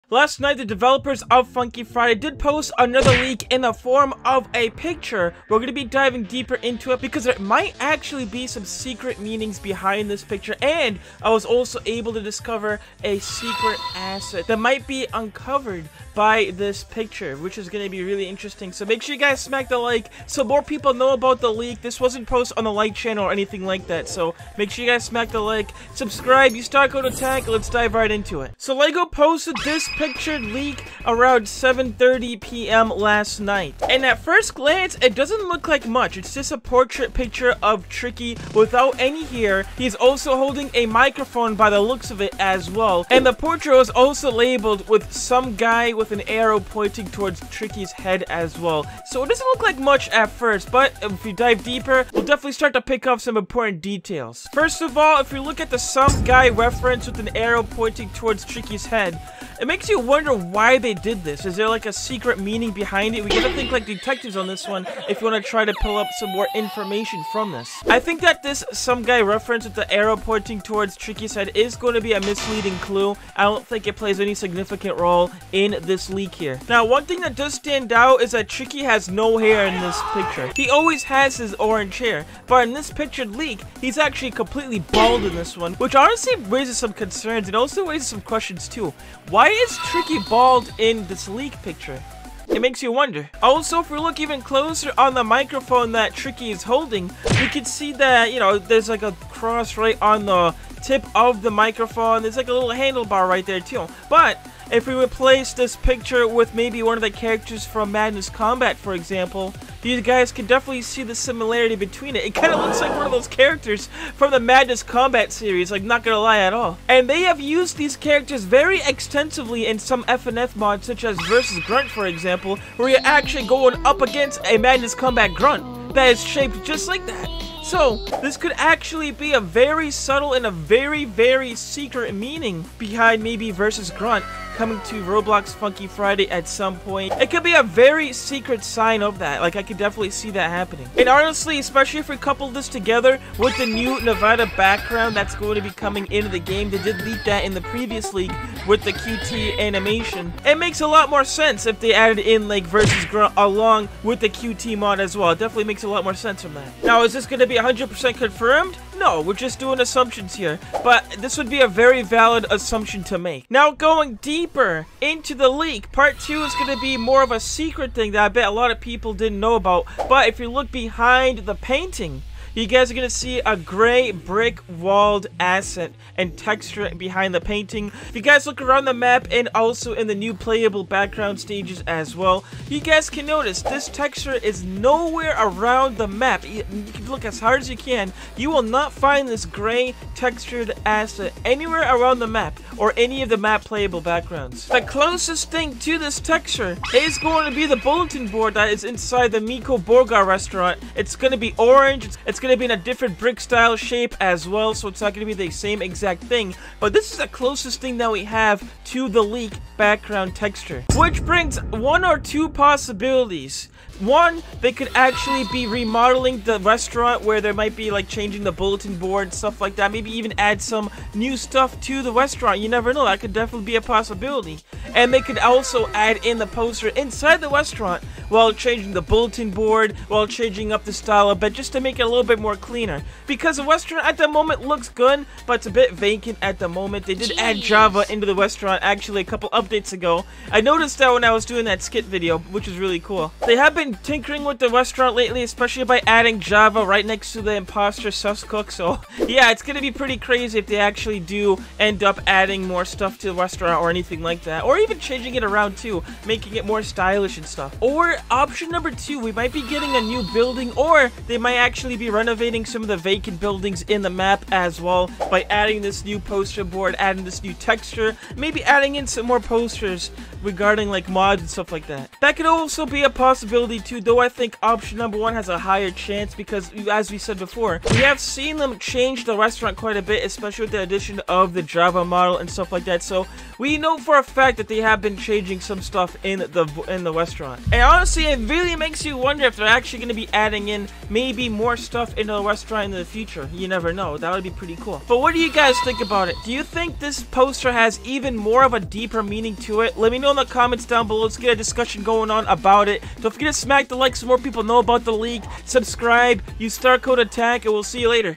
DEEP TRICKY LEAK! UNCOVERED SECRETS?! (Roblox Funky Friday)
Today we'll be diving deeper into the Tricky Poster leak for Roblox Funky Friday that was posted in the Funky Friday Discord server last night. There is actually some deep secrets you guys didn't know which may lead to a new Miko Borgar restaurant remodel?! Let's dive even deeper!

Timestamps
0:00 Intro
0:28 The Tricky Poster Leak
1:16 Why Is Tricky Bald?
1:32 VS Grunt Hint?!
2:43 New Textures Uncovered?! (2 SCENARIOS)
4:55 Outro & Discussion